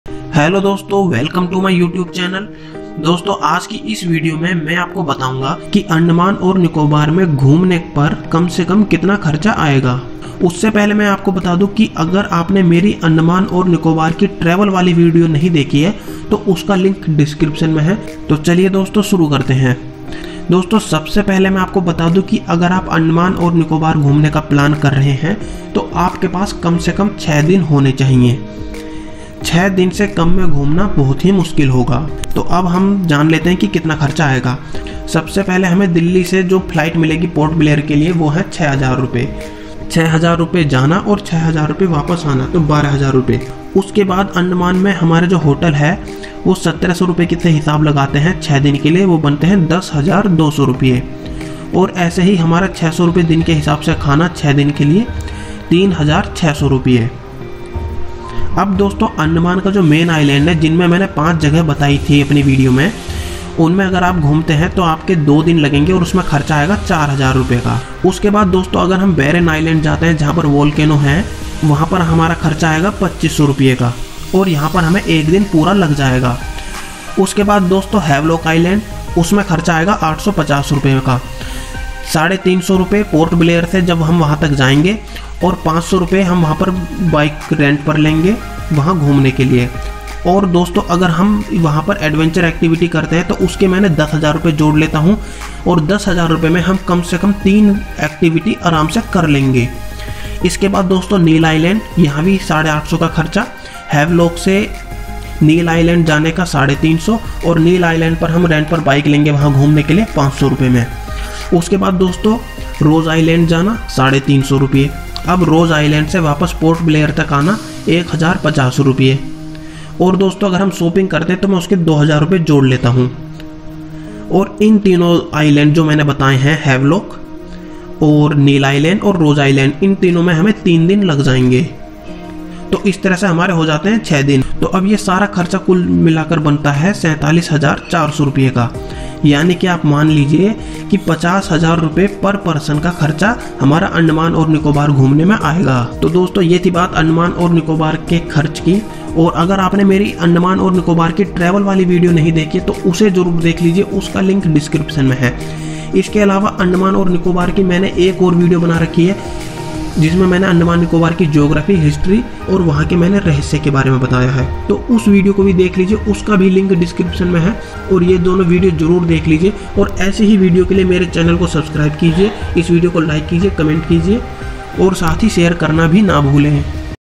Budget and Planning Of Andaman & Nicobar / Travel Expenses / Tourist / Tourism / Havelock Island
dosto mene is video me andaman & nicobar me minimum travel expenses btaya he ,,,,,,

agr aapka andaman & nicobar se juda koi swal he to aap comment me puch skte he ,,,